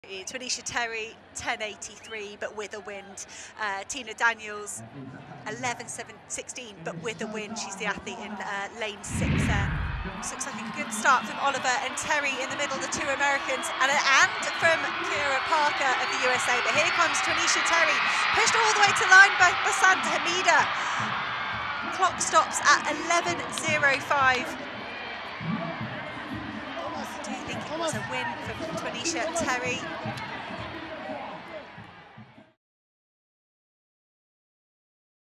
Terry victorious over 100m in Botswana | Continental Tour Gold 2023
You are watching a highlight clip of the Continental Tour Gold. Due to rights-restrictions we're unable to show full races on here, but you can find out where to watch the Continental Tour Gold in your territory